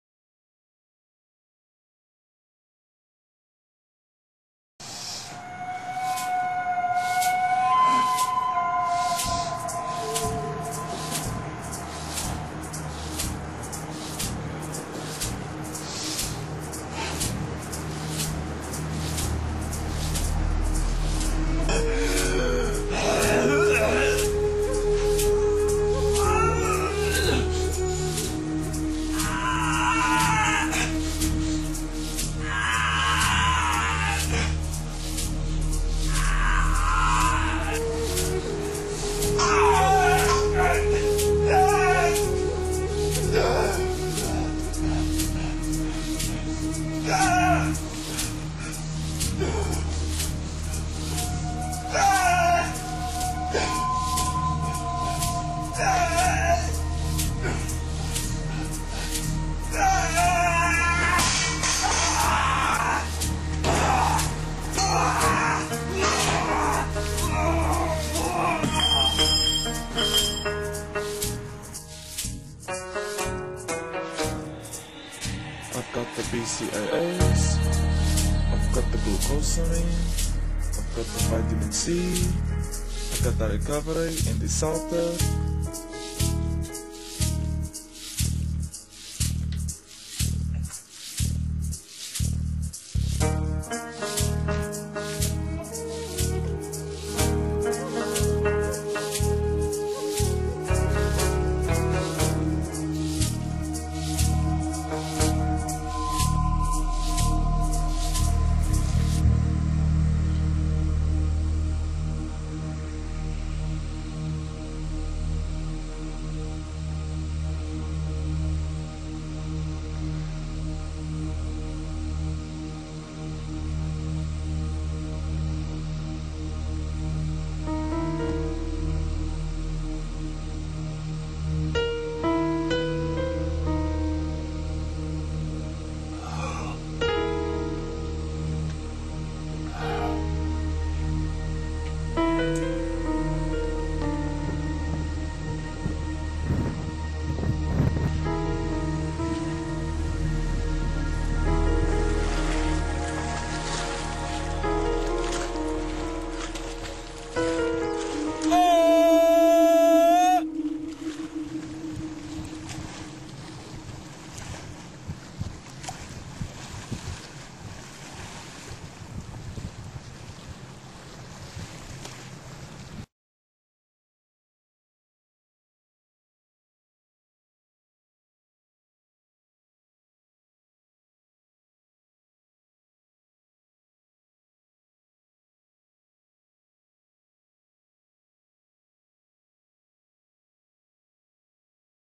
The Soul is Greater Than the World
This is the first video my friend and I made about the Swedish athlete Ricky Bruch.

Inspired by Ricky Bruch's "The Soul is Greater Than the World" Documentary. The video was shot over a 5-day period, and editing took about 2 days. Shot with the Sanyo Xacti CG6 camera.